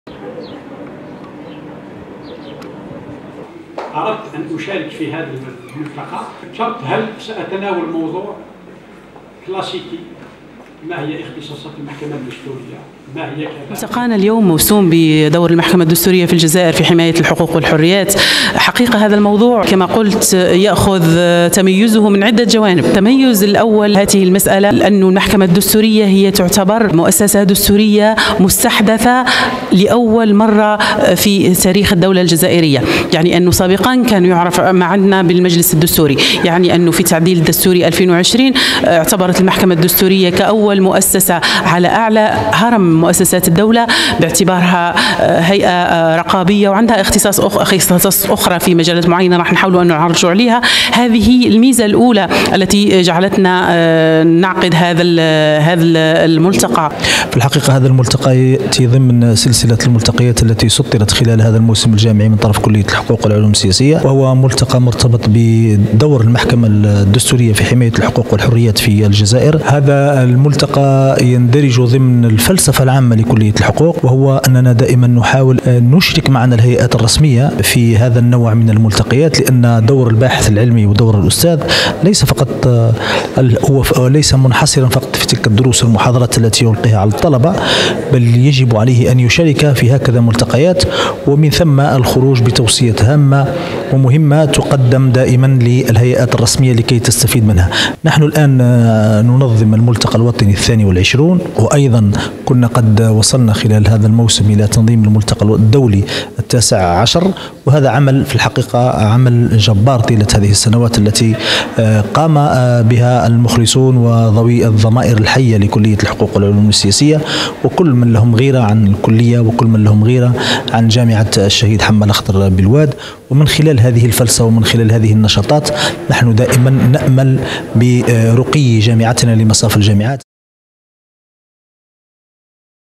الوادي / ملتقى حول دورالمحكمة الدستورية في حماية الحقوق والواجبات
المحكمة الدستورية والتي تعتبر مستحدثة في تاريخ التعديلات القانونية بالجزائر بصفتها كهيئة رقابية في حماية الحقوق والحريات هو محور هذا الملتقى مع قضايا اخرى متعلقة بتطوير  قطاع العدالة في الجزائر ،